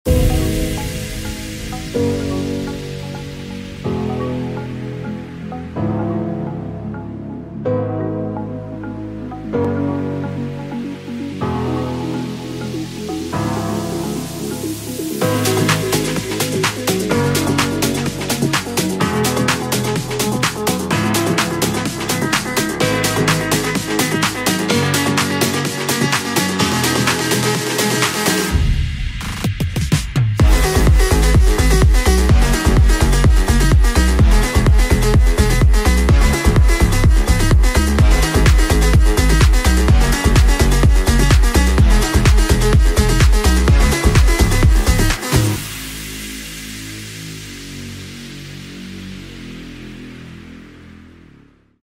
IPSA (Apothekers) 20j bestaan - Feathers Events in Café Local - invites Regi & Friends
In samenwerking met Jetset Events organiseerde Feathers Events het 20-jarig bestaan van IPSA in Café Local!